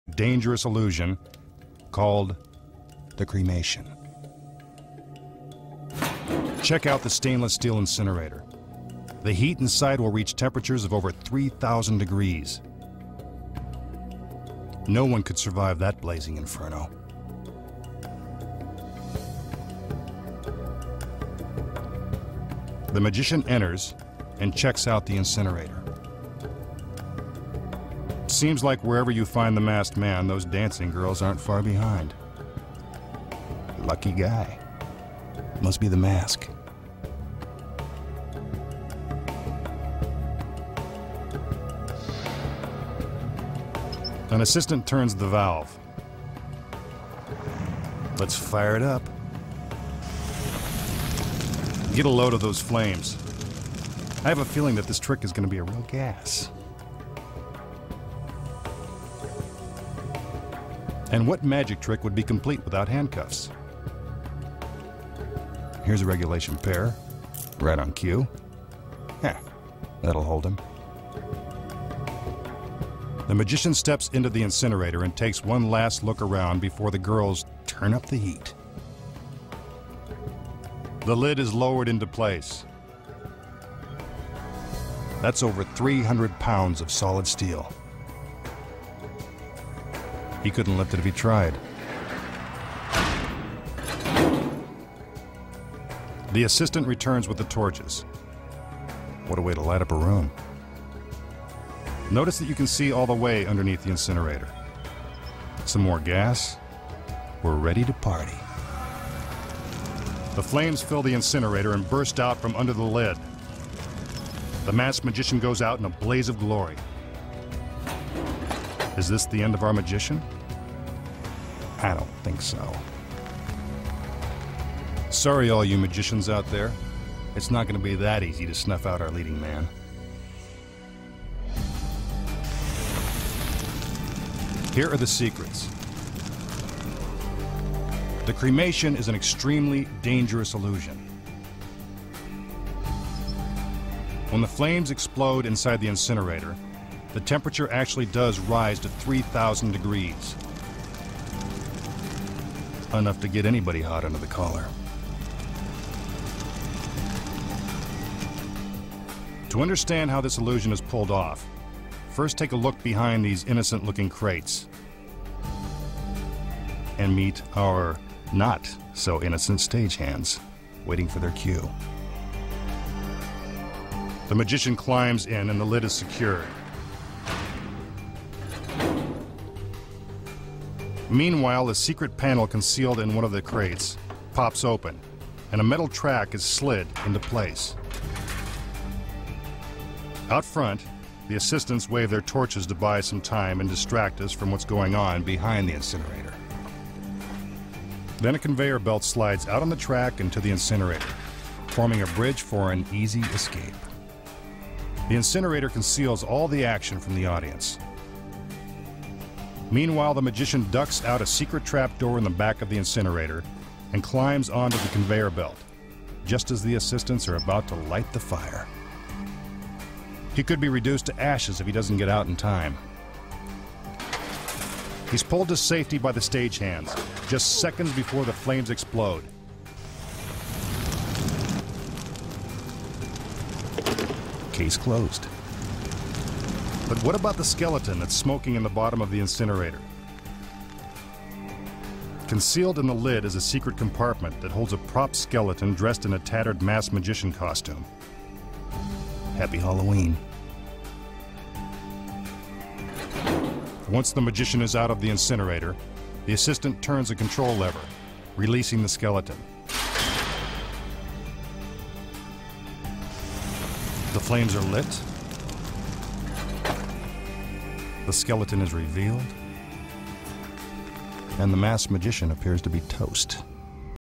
The cremation
Burnt alive n live to tell the secret behind this amazing trick